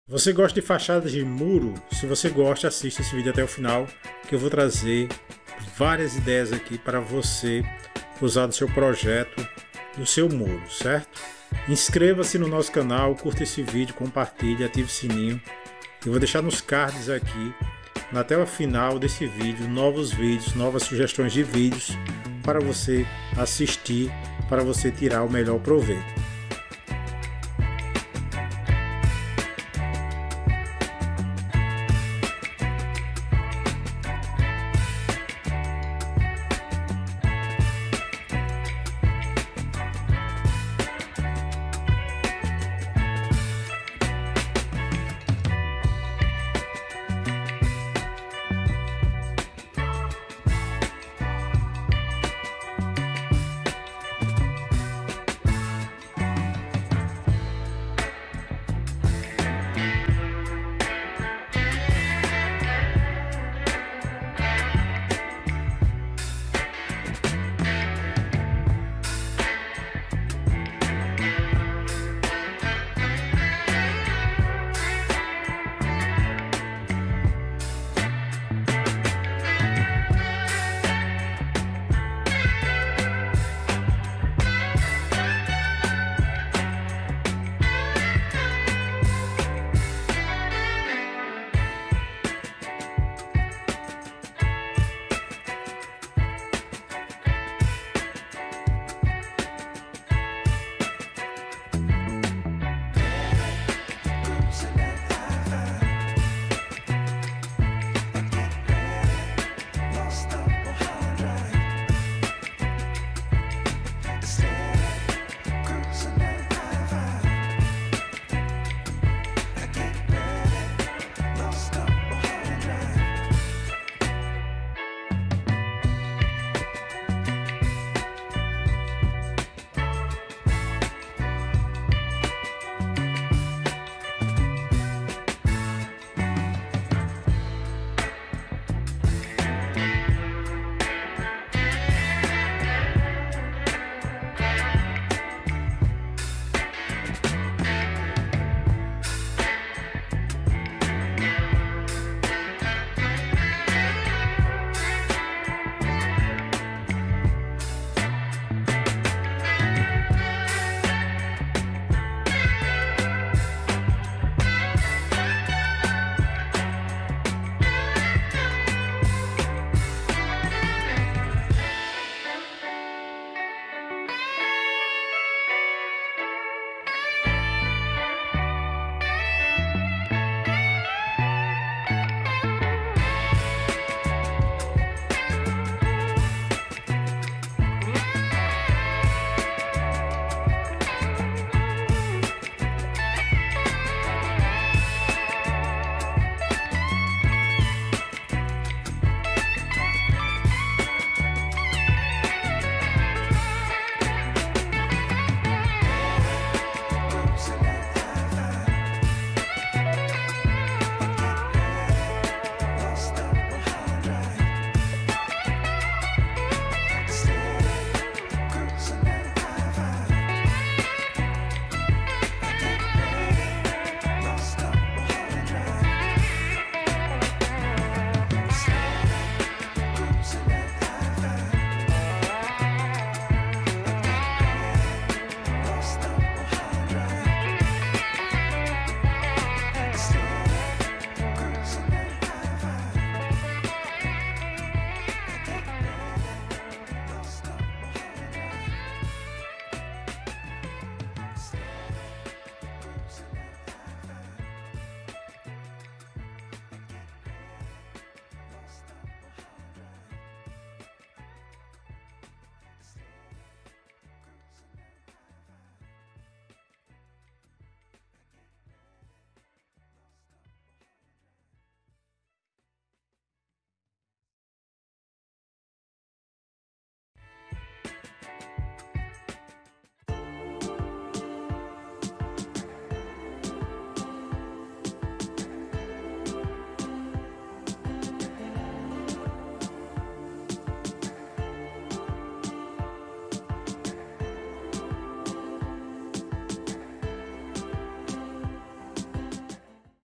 Fachadas de muros criativas para casas pequenas ideias para maximizar o espaço
Se você tem uma casa pequena e quer maximizar o espaço, uma das maneiras de fazer isso é com uma fachada de muro criativa. Além de trazer um toque de personalidade para sua casa, as fachadas criativas de muros podem ajudar a aproveitar melhor o espaço externo e interno.

Neste vídeo, mostraremos 10 ideias de fachadas de muros criativas para casas pequenas, que podem inspirar você a transformar a aparência da sua casa e maximizar o espaço disponível.

Desde fachadas com jardins verticais até paredes com texturas e materiais diferentes, você encontrará diversas ideias criativas para dar uma nova cara à sua casa. Além disso, mostraremos como essas fachadas podem ser combinadas com outros elementos decorativos para criar um ambiente ainda mais acolhedor e funcional.

Não perca tempo e confira nossas dicas para ter uma fachada de muro criativa e funcional para a sua casa!

Decoração Para Jardim

Decoração Com Madeira

Fachadas de Muro

Fachadas de Casas

☑️ Isenção de responsabilidade:
Os métodos mostrados são de sites já disponíveis na internet.